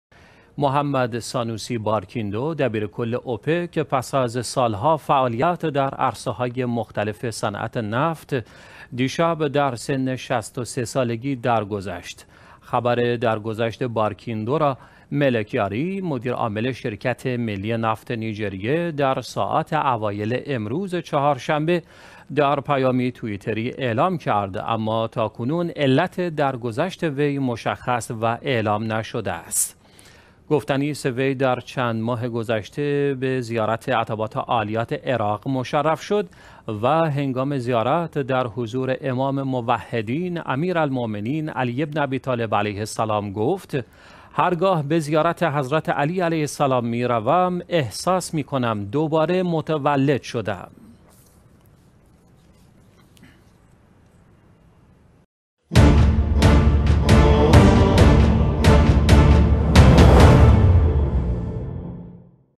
دبیرکل اوپک درگذشت
«محمد سانوسی بارکیندو» دبیرکل اوپک پس از سال‌ها فعالیت در عرصه‌های مختلف صنعت نفت، دیشب در سن ۶۳ سالگی درگذشت.

خبر درگذشت بارکیندو را «مله کیاری» مدیر عامل شرکت ملی نفت نیجریه در ساعات اوایل امروز چهارشنبه در پیامی توئیتری اعلام کرد اما تاکنون علت درگذشت وی مشخص و اعلام نشده است.

گفتنی است، وی در چند ماه گذشته به زیارت عتبات عالیات عراق مشرف شد و هنگام زیارت در حضور امام موحدین امیرالمؤمنین علی بن ابی طالب علیه السلام گفت: هرگاه به زیارت حضرت علی علیه السلام می روم، احساس می کنم دوباره متولد شده ام.